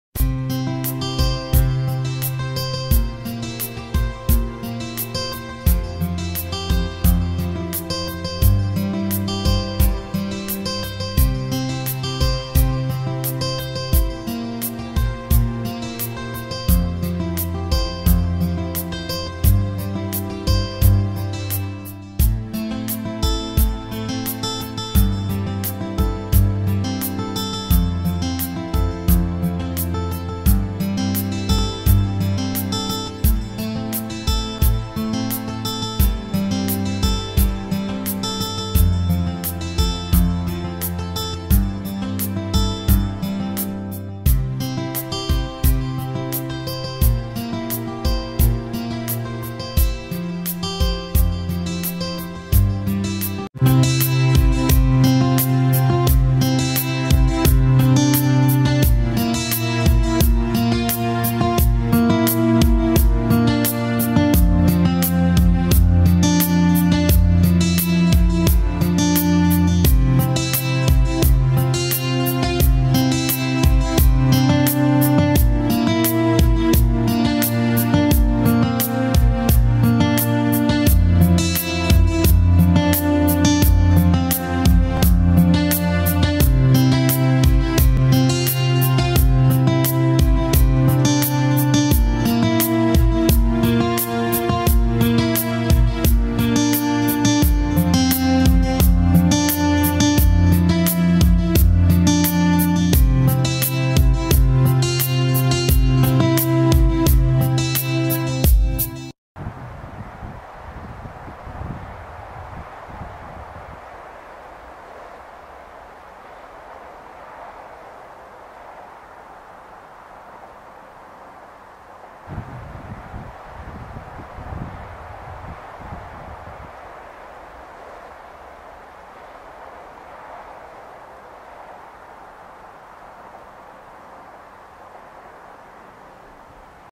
Bald Eagles
Bald eagles winter fishing near Red Wing, MN